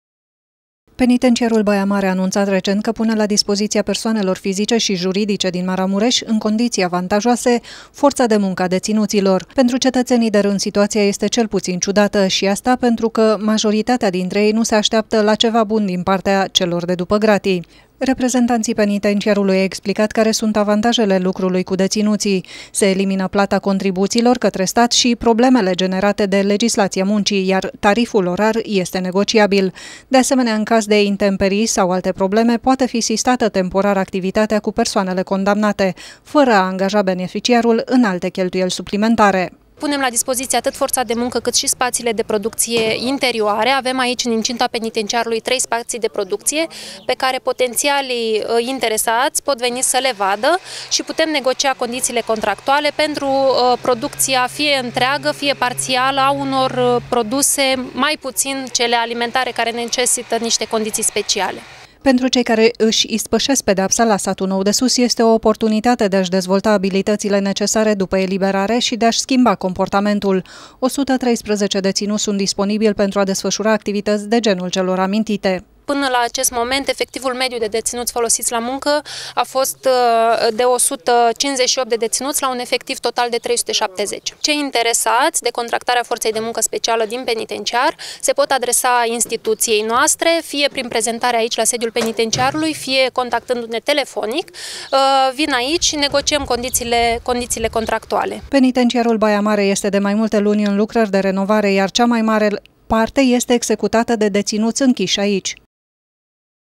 DEŢINUŢII, FORŢĂ DE MUNCĂ IEFTINĂ
Situatie inedita  la Penitenciarul Baia Mare. Institutia de detentie pune la dispozitia maramuresenilor, forta de munca a celor incarcerati la Satu Nou de Sus.